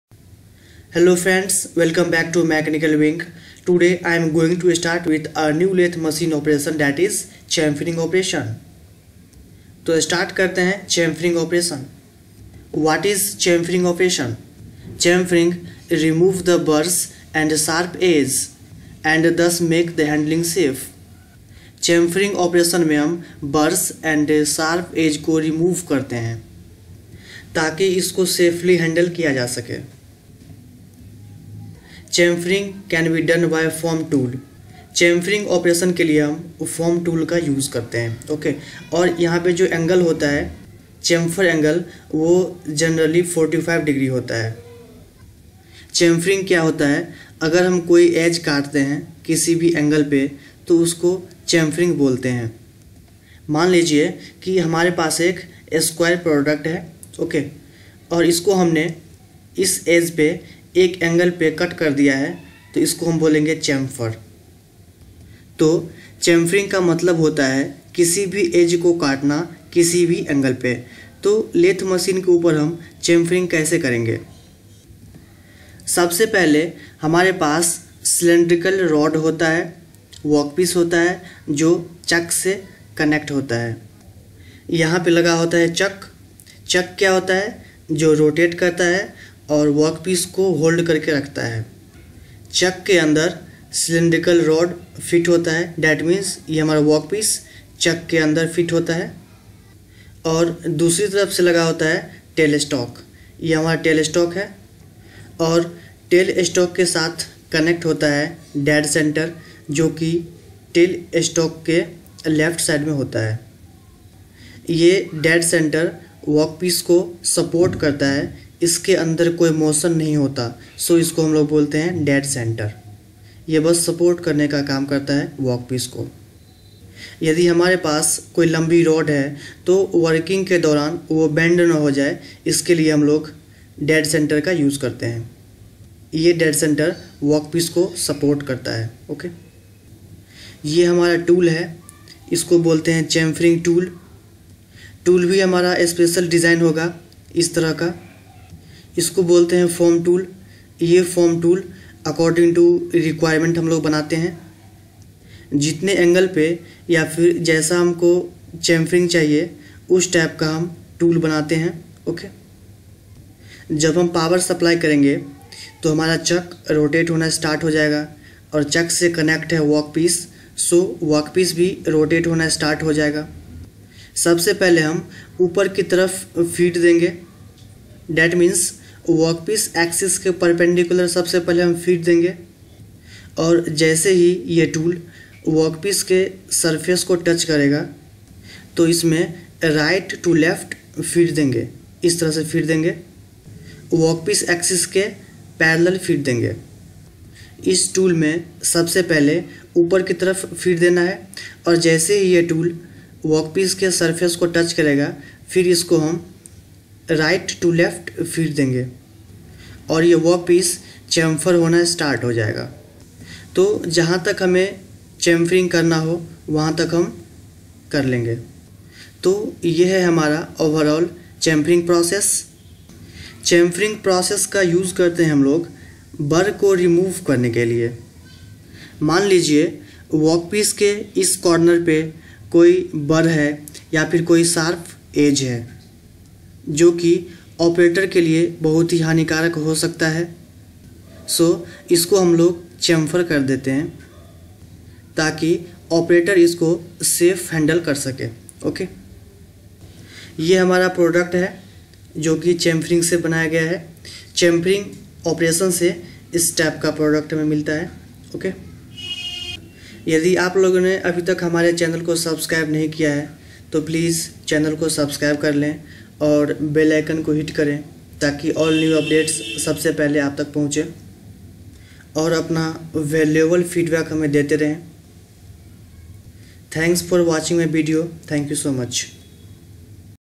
Machining Operation Lecture -8 Chamfering Operation (Hindi & English).
chamfering operation...
lathe machine operation.